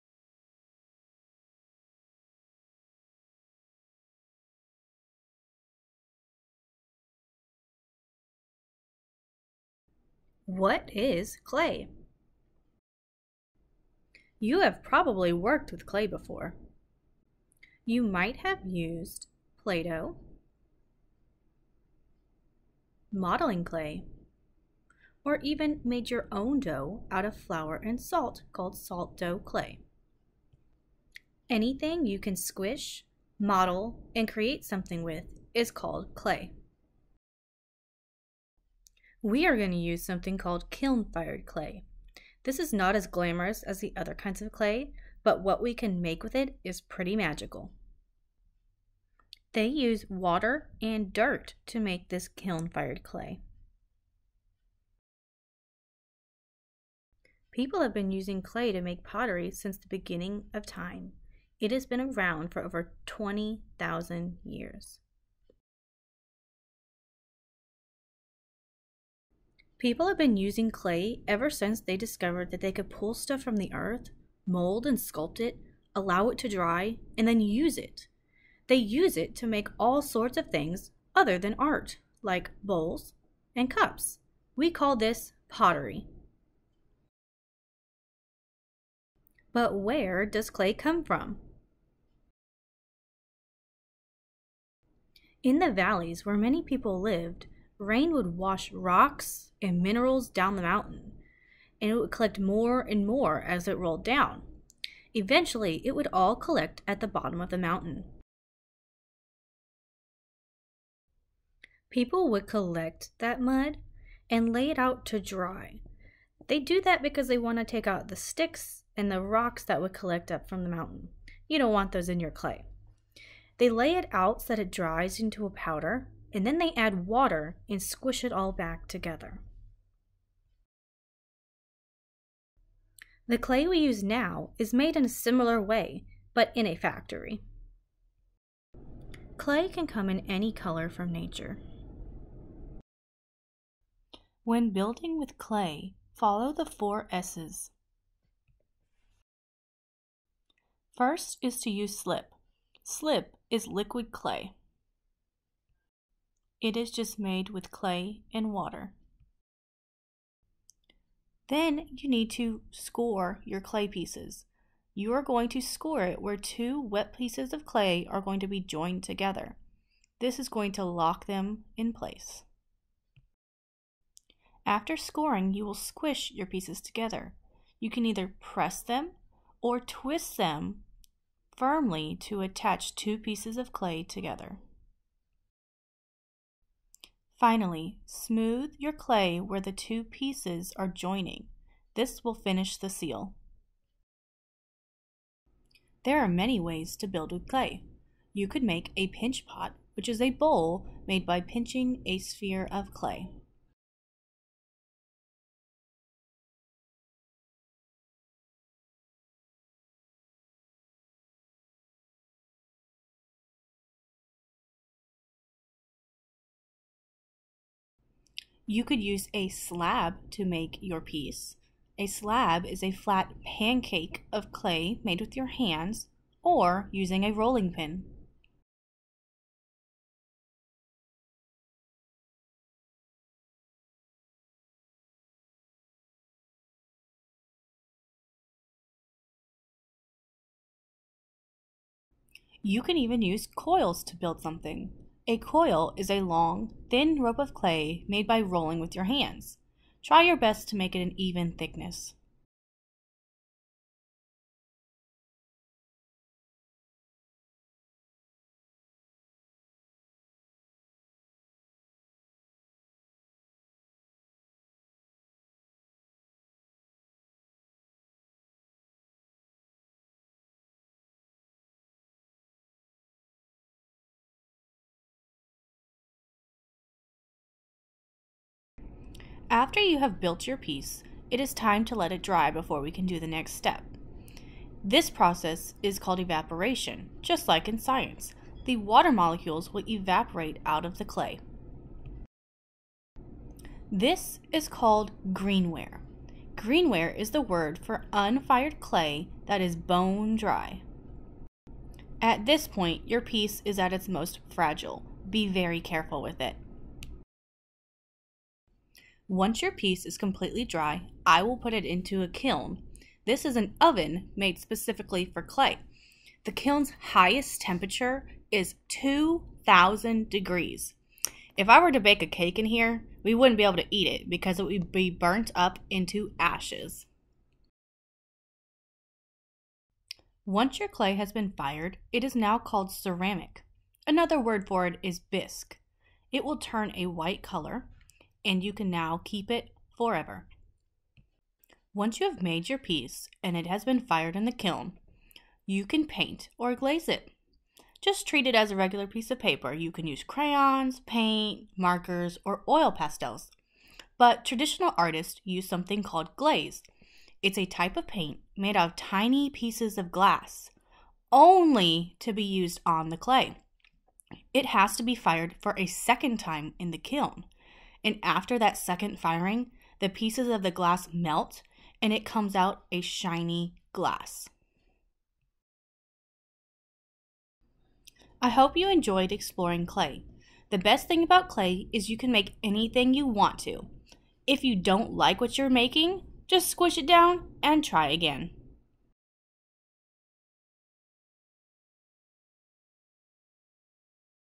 What Is Clay
What is clay? Where does clay come from? In this video, I explain a little about what clay is, where it comes from, and the process of making, firing, and painting the clay. More videos on projects to make with clay to come.

These videos were created for use in my classroom.